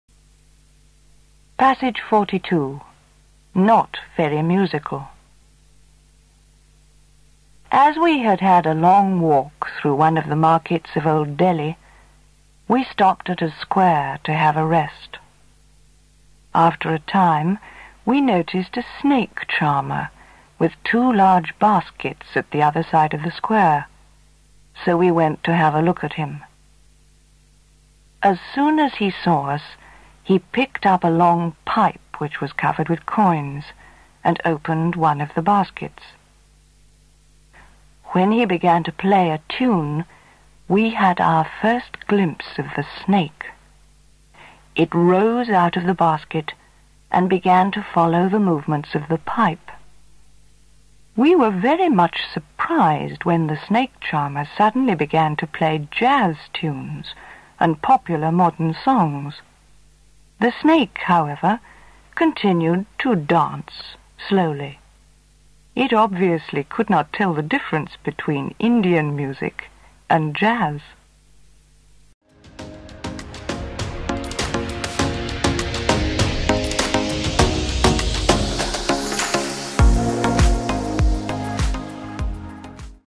NOT VERY MUSICAL - NEW CONCEPT ENGLISH 2 - PRACTICE AND PROGRESS
NOT VERY MUSICAL is one of many series in New Concept English 2, practice and progress, here is the story.

Not very musical

As we had had a long walk through one of the markets of Old Delhi, we stopped at a square to have a rest.
After a time, we noticed a snake-charmer with two large baskets at the other side of the square, so we went to have a look at him.
As soon as he saw us, he picked up a long pipe which was covered with coins and opened one of the baskets.
When he began to play a tune, we had our first glimpse of the snake.
It rose out of the basket and began to follow the movements of the pipe.
We were very much surprised when the snake charmer suddenly began to play jazz tunes and modern pop songs.
The snake, however, continued to 'dance' slowly.
It obviously could not tell the difference between Indian music and jazz!

Audio n story by: L.G. Alexander